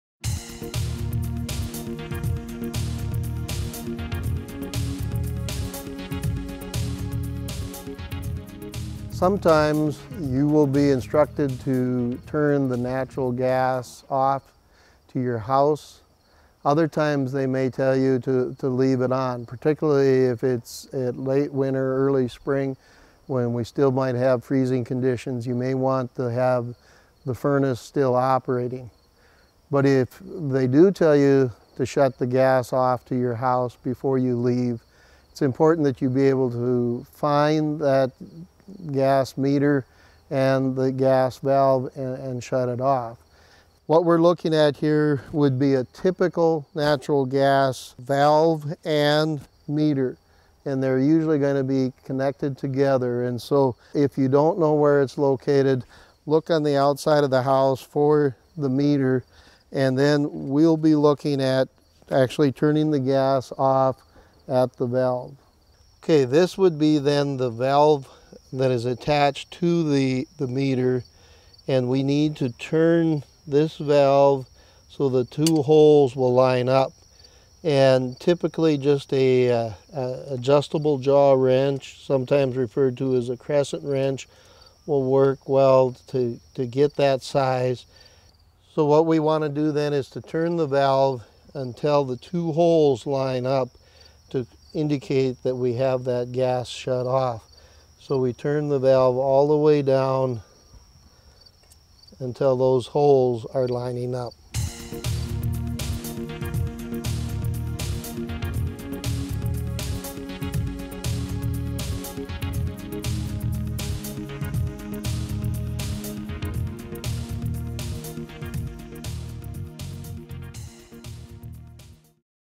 Preparing to evacuate your home when flooding is imminent: Turning off natural gas
Gas appliances will not function properly in a flooded home, which may lead to a gas accumulation and a fire hazard. The video describes the procedure for turning off the natural gas coming to a house.